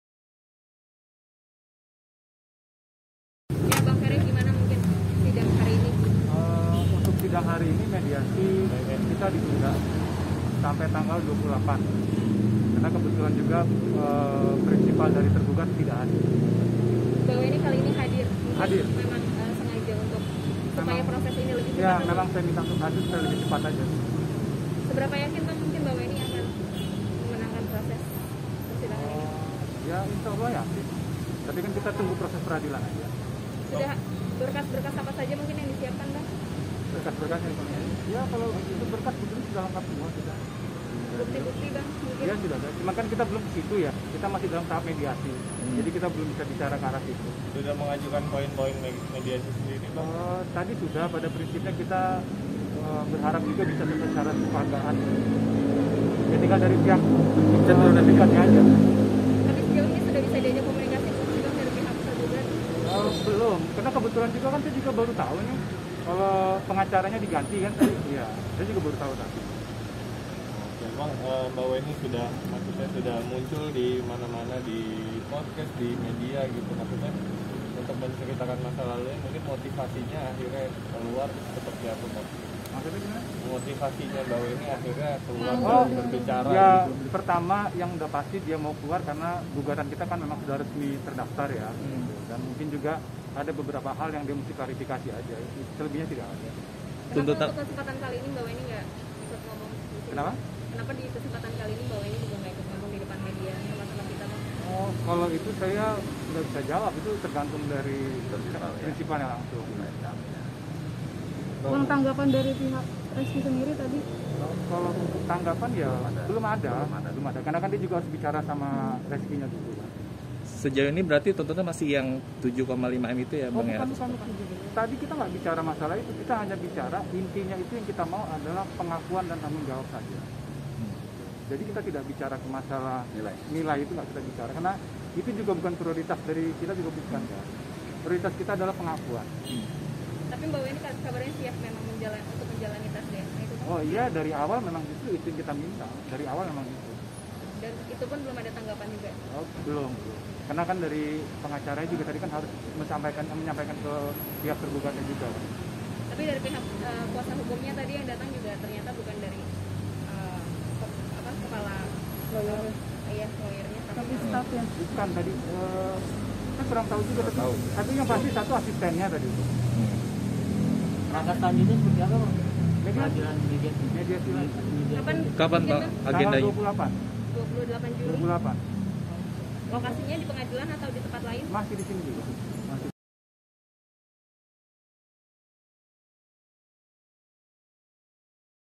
FERRY ASWAN, S.H., M.A. KUASA HUKUM WENNY ARIANI, ... TUNTUTAN PENGAKUAN & TANGGUNG JAWAB
Jakarta, 14 Juli 2021

TANPA TINTA
MENGHADIRKAN BERITA TANPA TINTA SECARA FAKTA DAN TERPERCAYA YANG DIKEMAS SECARA EKSENTRIK TANPA TRIK.